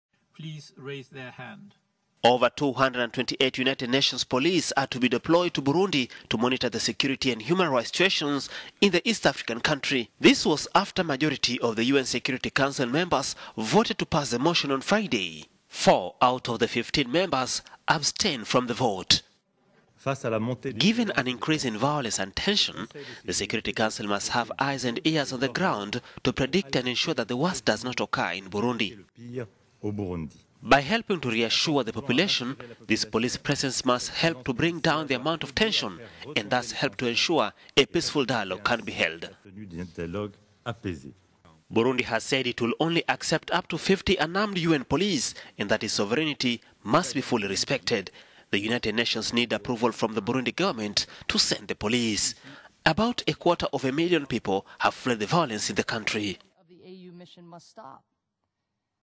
U.N. Security Council approves police deployment to Burundi
Over 228 United Nation police are to be deployed to Burundi to monitor the security and human rights situation in the east African country.

This was after majority of the UN Security Council members voted to pass the motion on Friday. Four out of the 15 member-body abstained from the vote.

According to Reuters, more than 450 people have been killed since President Pierre Nkurunziza pursued and won a third term last year, a move his opponents say violated the constitution and a peace deal that …